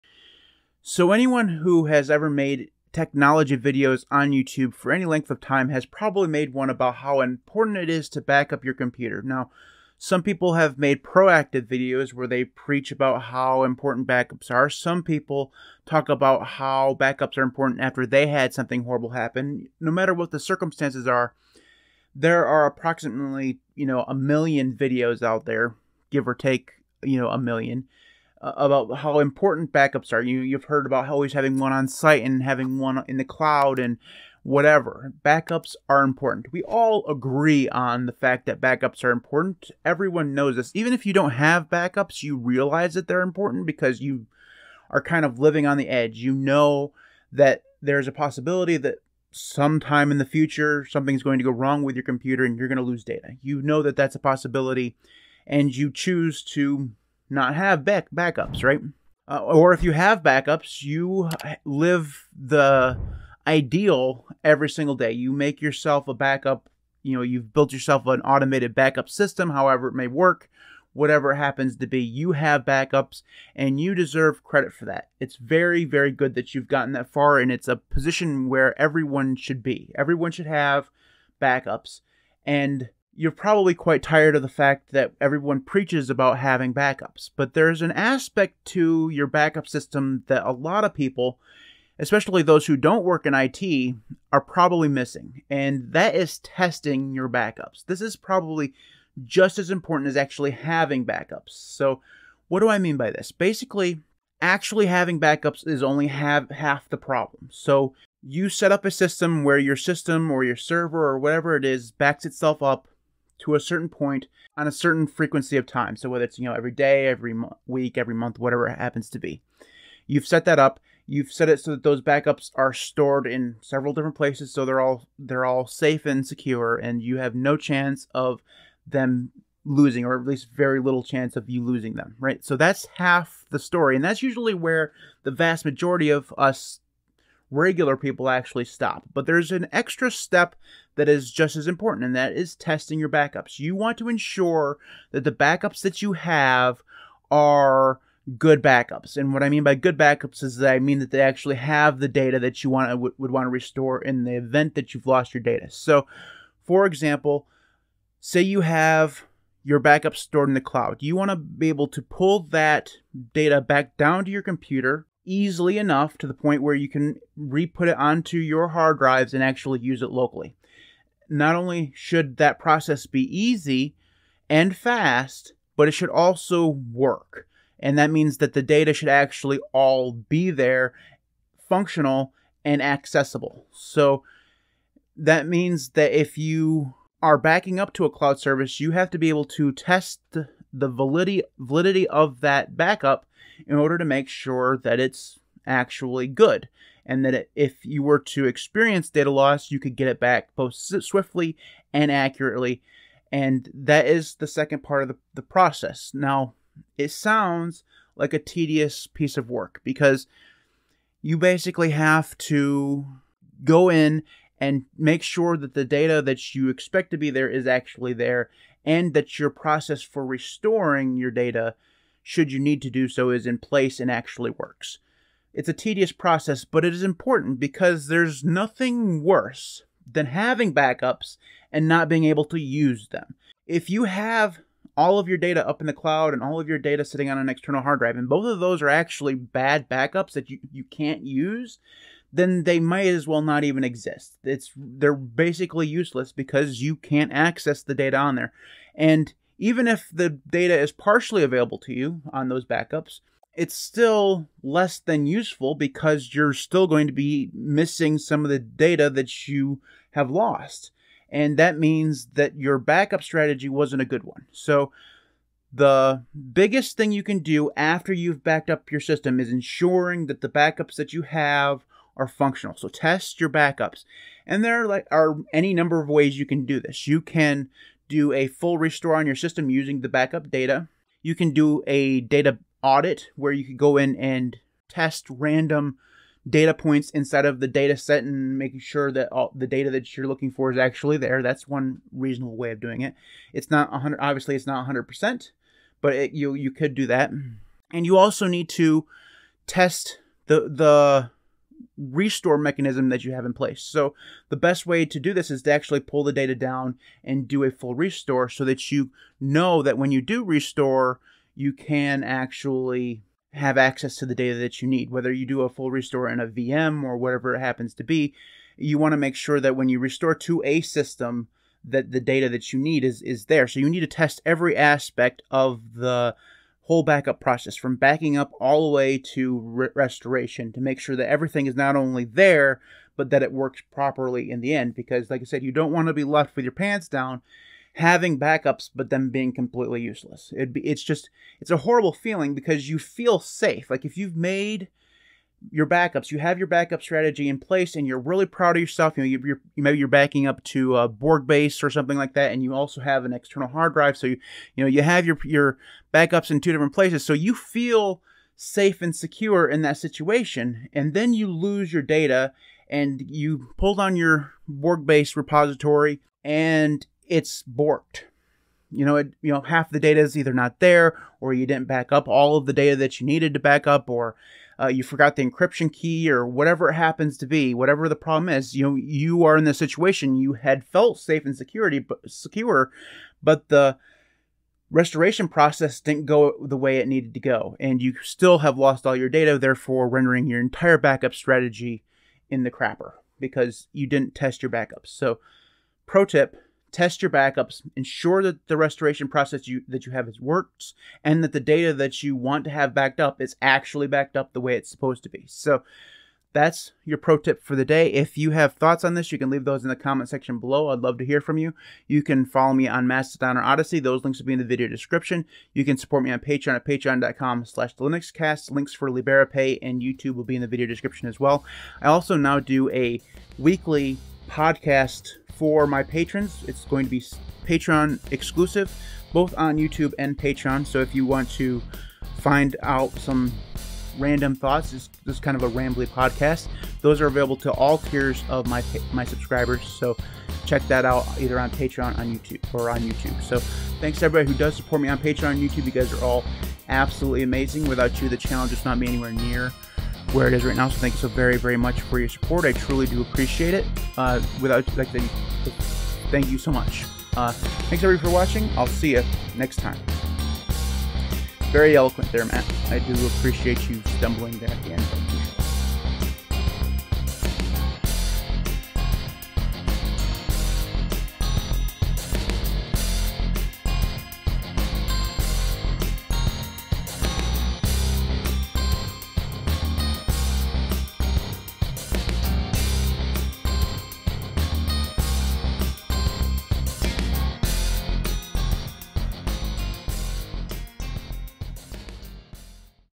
Verify Your Backups: It Could Save Your Life!
Today I talk about why your backup strategy is missing a crucial step.